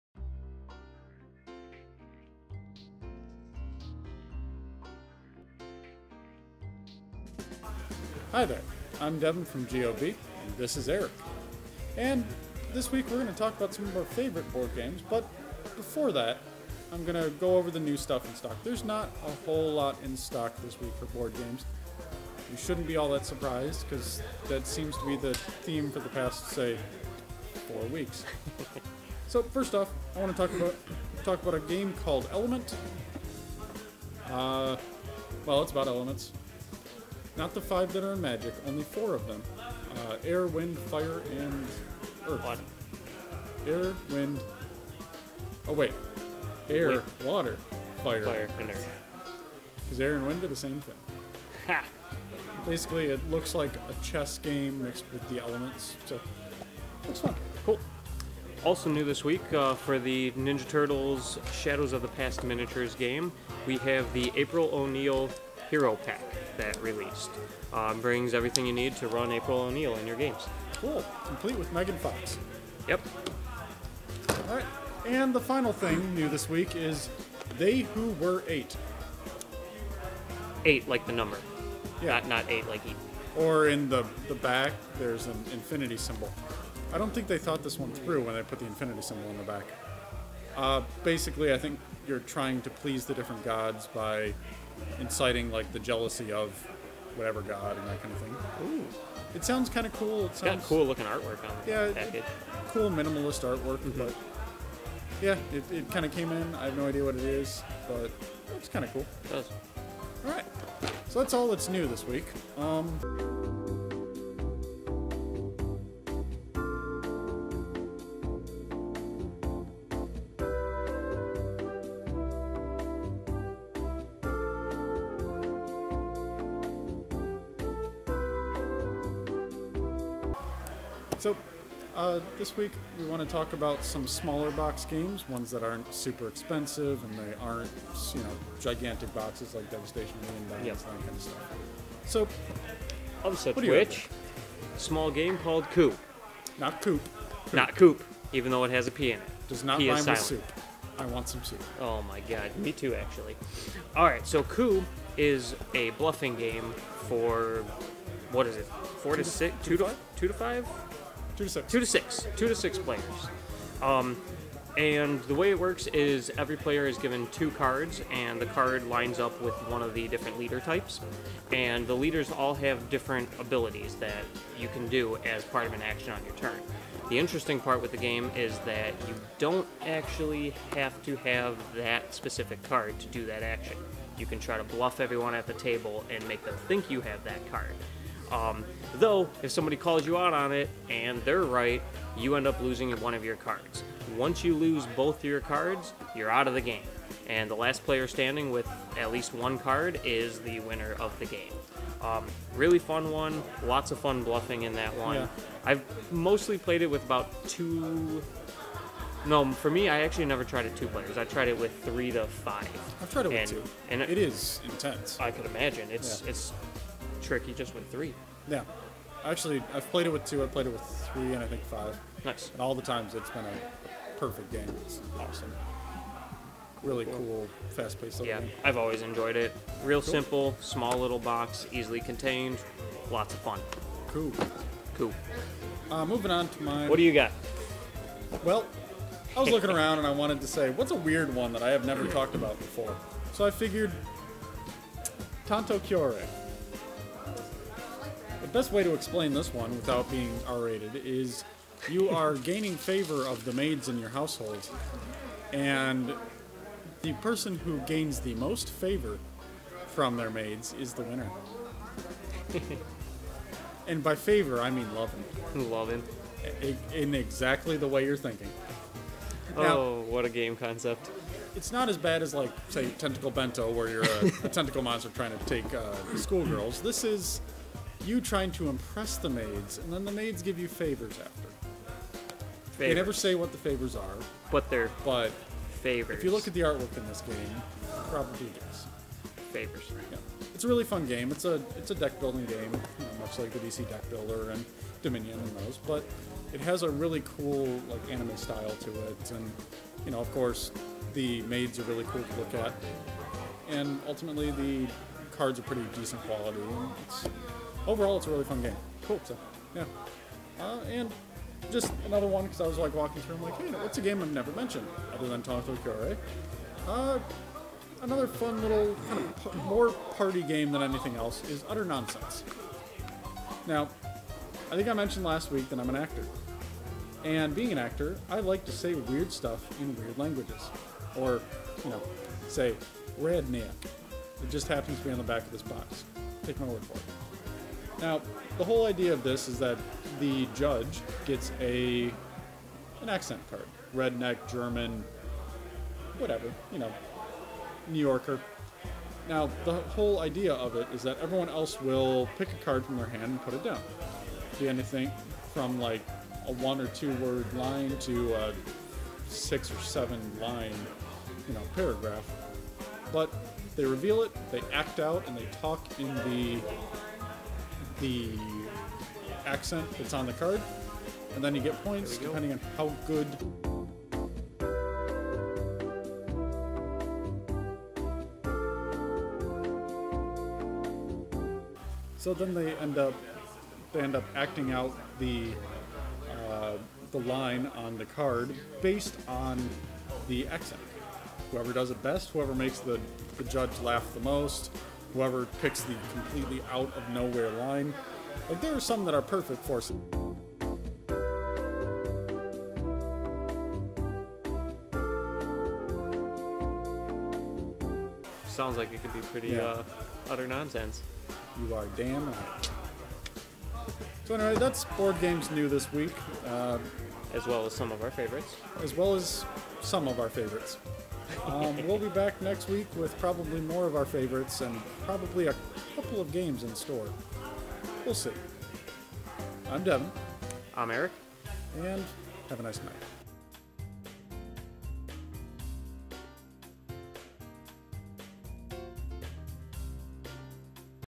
This Week In Board Games 3-15-17
Devon and Erik battle a light release schedule and people walking in front of the camera to talk to us about Element, Coup, Tanto Cuore, and more!